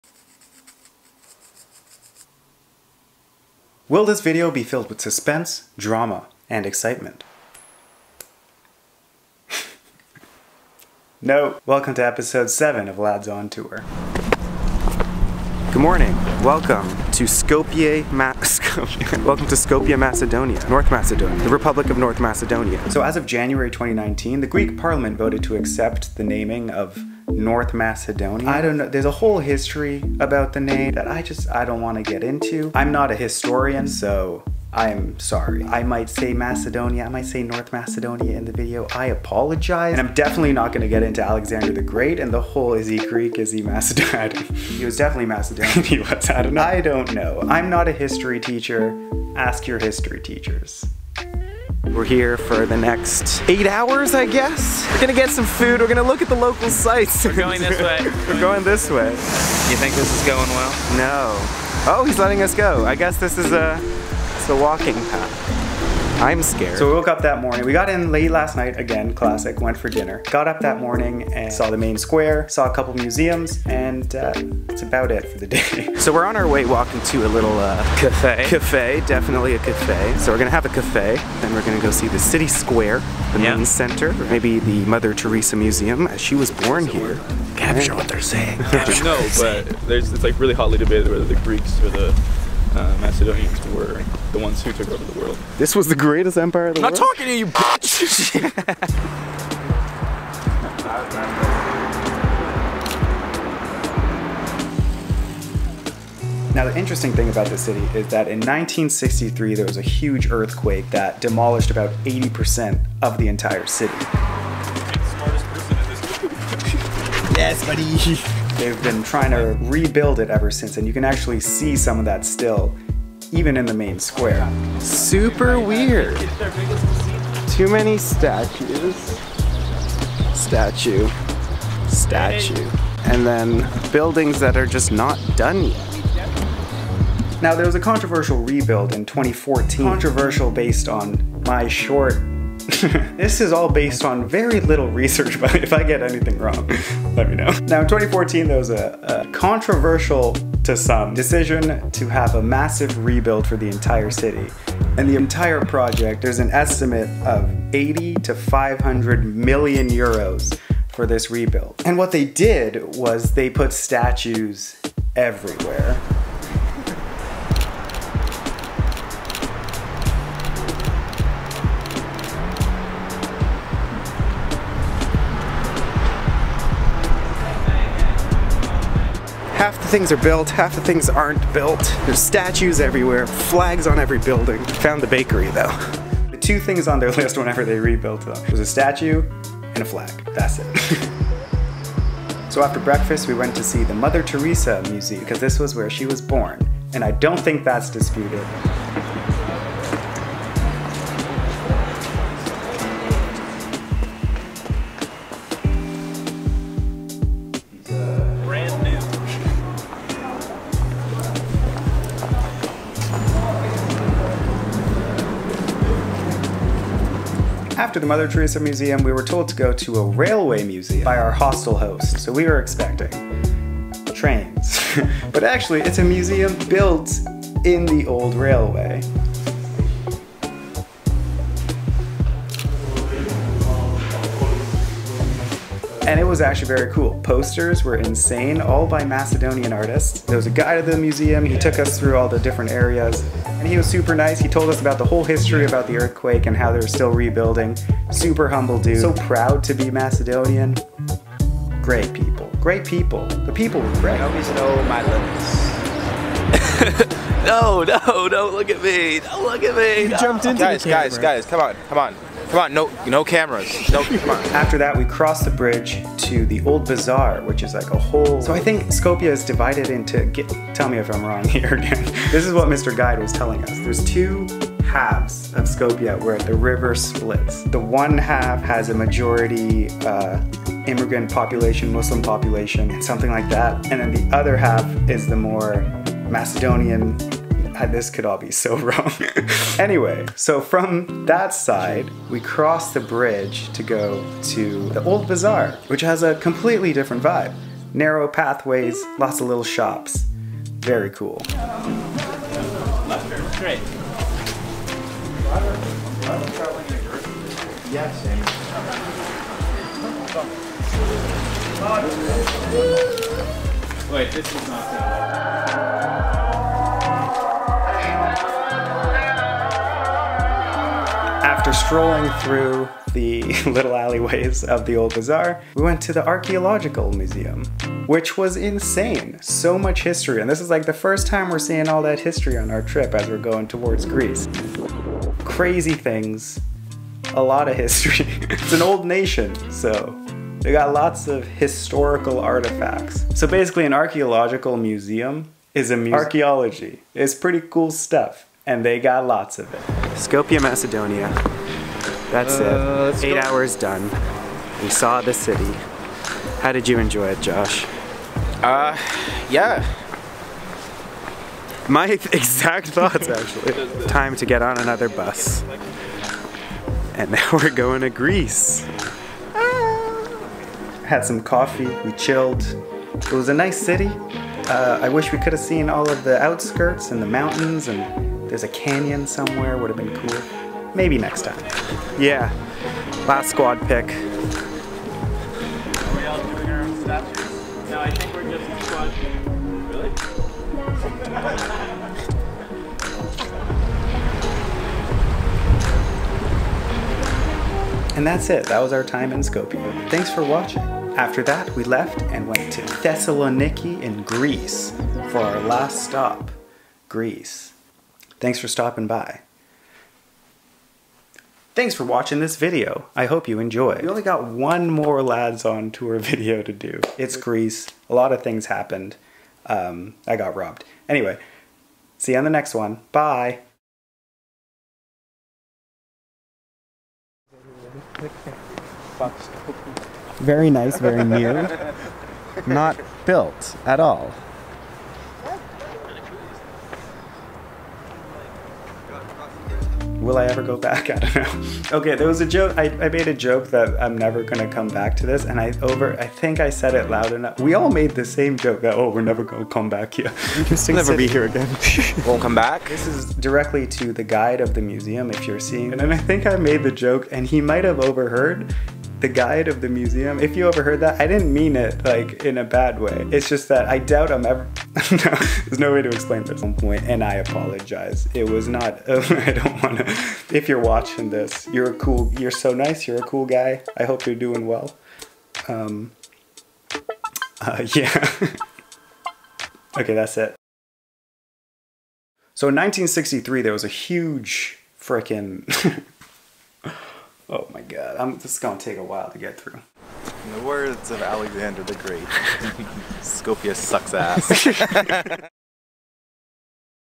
8 Hours in Skopje, Macedonia | The City of Statues
The lads have made it to Skopje, North Macedonia. This one is very chill as i poorly explain a little bit of the history and what the city is all about. we hung out, walked around, went to a few museums, and enjoyed our short stay.

Feb 2020
7/8

Music by an artist I follow: Aldo , from his Random Jam series
Follow him, hes dope:
bop^

presented in 70mm welcome to the cinema